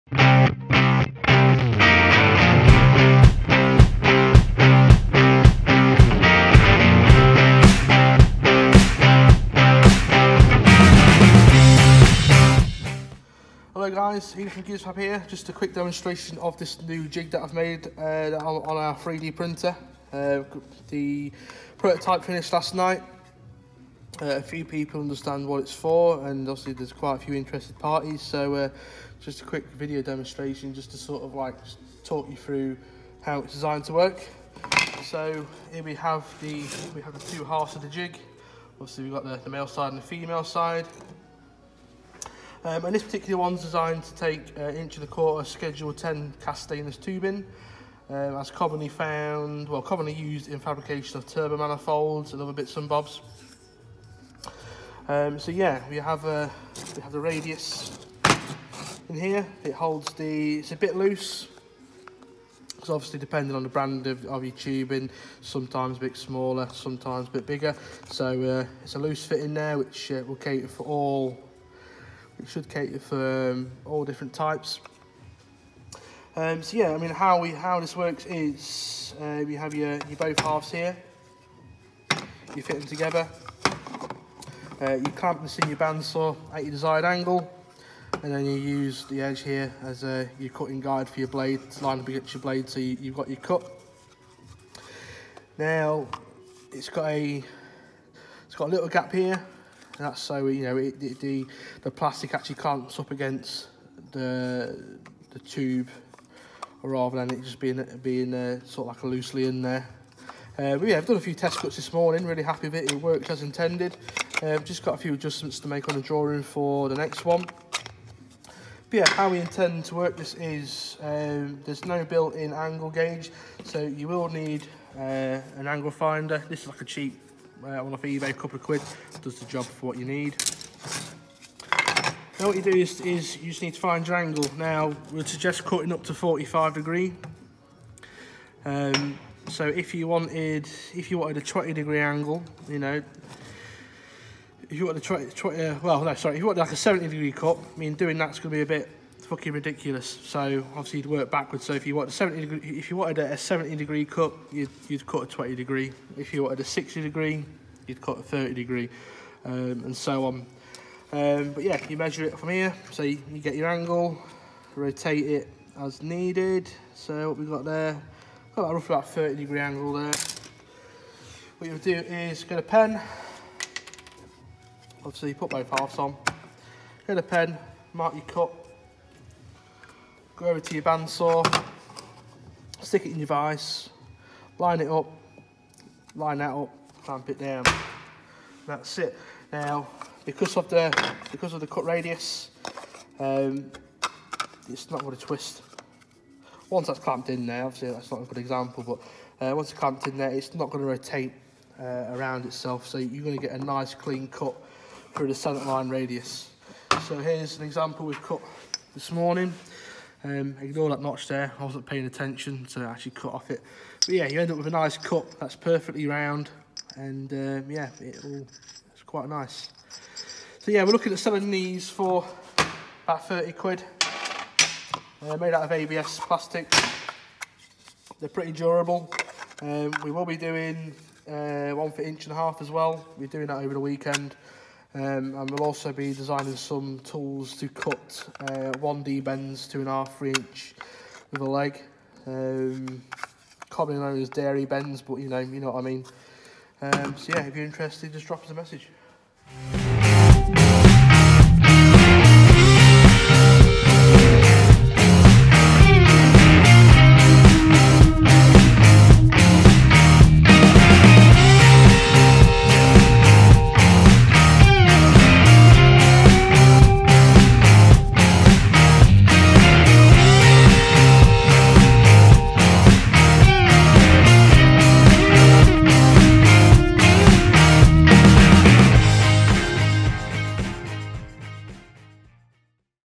SCH10 Manifold Elbow Cutting Fixture
Introducing one of our new products.

This small jig is designed to hold SCH10 Long Radius cast stainless elbows, firmly in your bandsaw vice for cutting.

These are 3D printed in house and we have options for 1.25" and 1.5" SCH10 and will firmly hold your elbow in your bandsaw vice for cutting.

Priced as just £24.99 ($33) its a tool you cannot afford not to have!